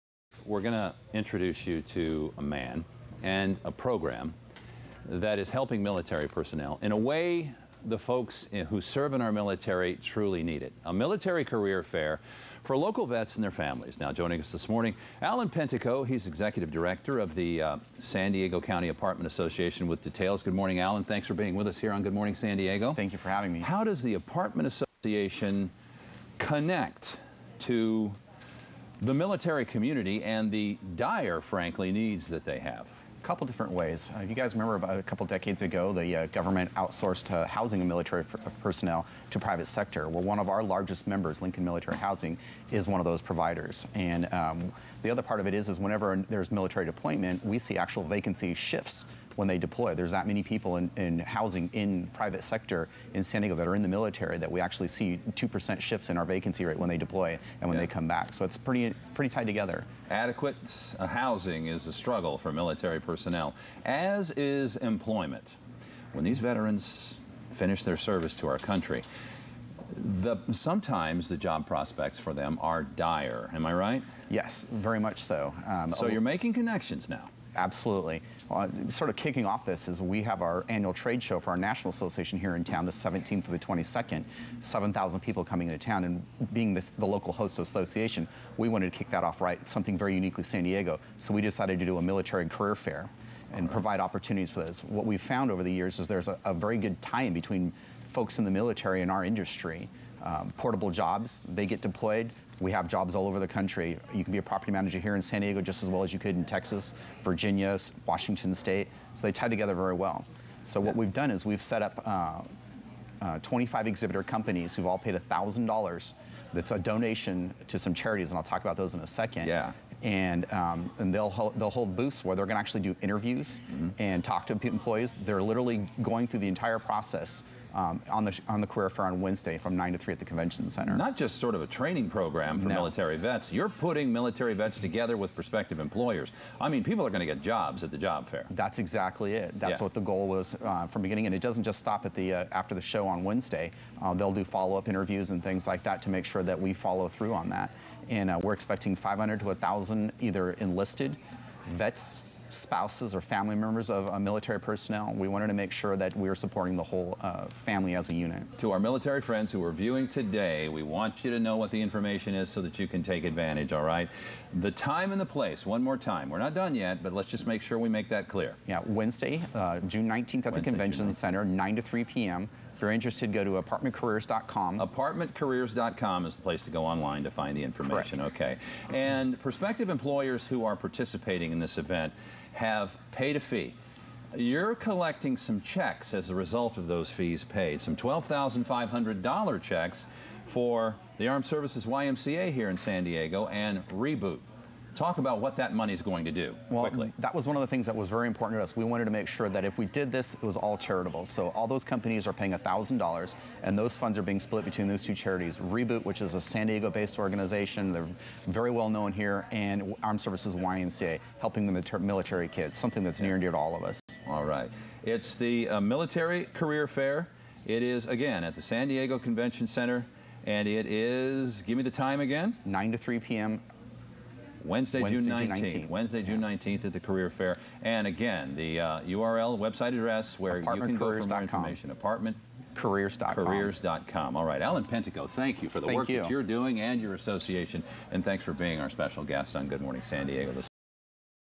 MILITARY CAREER FAIR KUSI TV 6 17 13 9am
The National Apartment Association collected $1000 from each of 25 vendors participating in a military career fair for two charities, NVTSI REBOOT and the Armed Services YMCA. Reporter Jon Du Pre interviews Alan Pentico with NAA about the career fair and its charitable support.